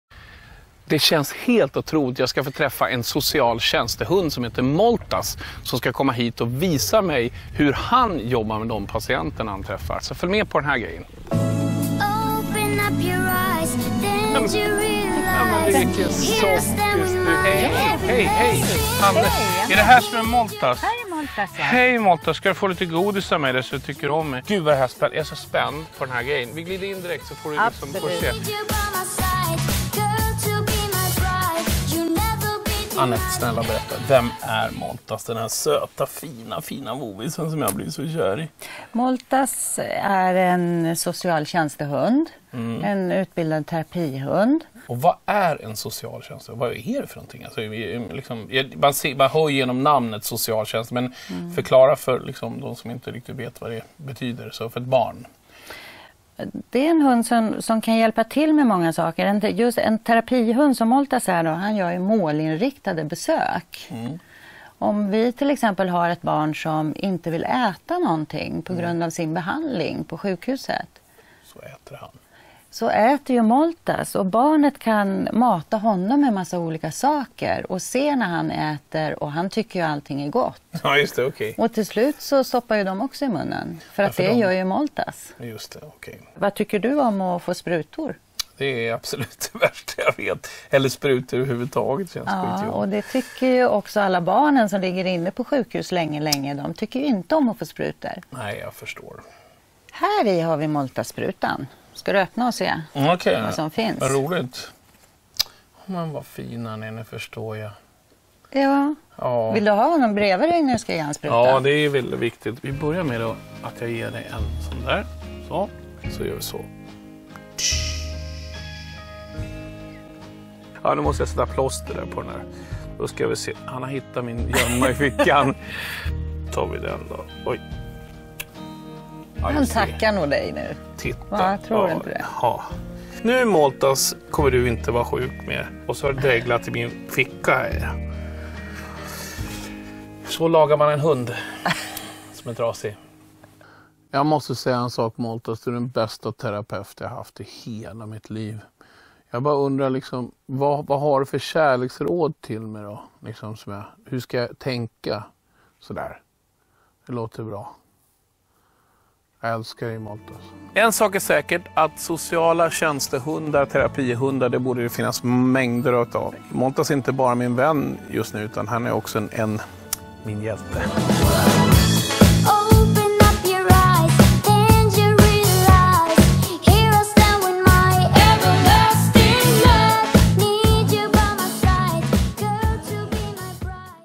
Anders Bagge träffar terapihunden Moltas
Certifierade terapihunden Moltas förgyller vardagen för barn på sjukhus tillsammans med sin matte Anette. Eftersom vi på Agria Djurförsäkring jobbar en del med Anders Bagge tog vi med honom på en liten rundtur i djursverige. Vi lät honom bland annat träffa hjälten Moltas!